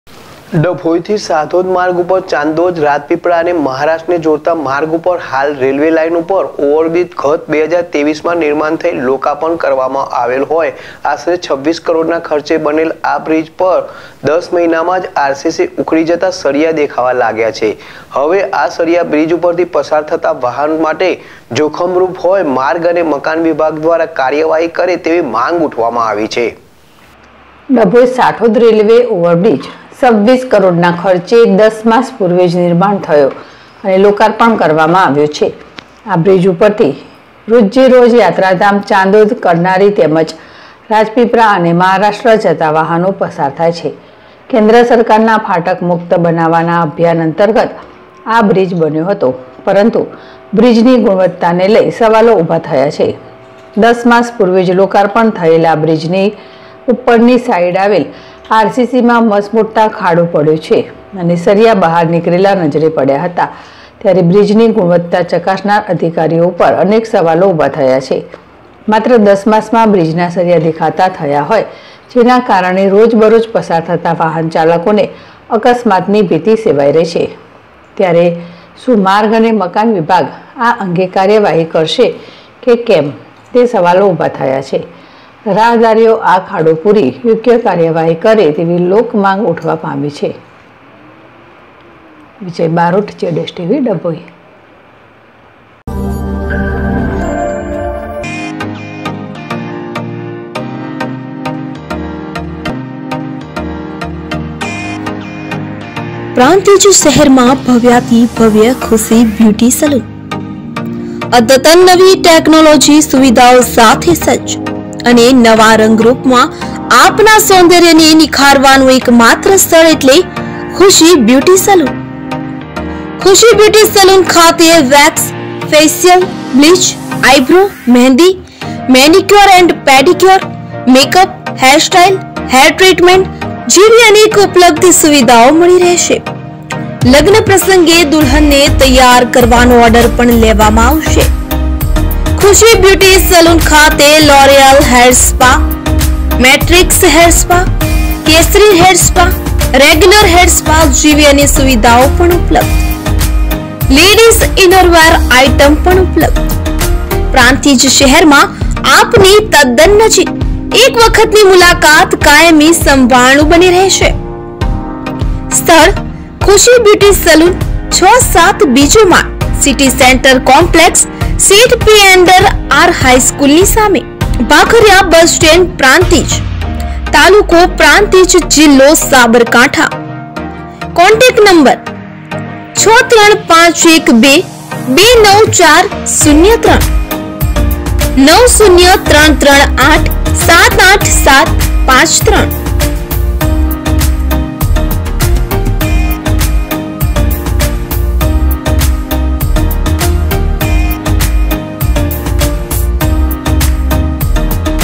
ડભોઇ થી સાઠોદ માર્ગ પર ચાંદોદ રાજપીપળા, અને મહારાષ્ટ્ર ને જોડતા માર્ગ ઉપર હાલમાં રેલ્વે લાઈન ઉપર ...
ડભોઇ થી સાઠોદ માર્ગ પર ચાંદોદ રાજપીપળા, અને મહારાષ્ટ્ર ને જોડતા માર્ગ ઉપર હાલમાં રેલ્વે લાઈન ઉપર ઓવર બ્રિજ ગત મે 2023માં નિર્માણ થઈ લોકાર્પણ કરવામાં આવ્યો હોય આશરે 26 કરોડના ખર્ચે બનેલ આ બ્રિજ પર 10 મહિનામાં જ આર.સી.સી ઉખડી જતા સળિયા દેખાવા લાગ્યા છે હવે આં સળિયા બ્રિજ પર થી પસાર થતા વાહનો માટે જોખમ રૂપ હોય માર્ગ અને મકાન વિભાગ કાર્યવાહી કરે તેવી માંગ ઉઠી છે.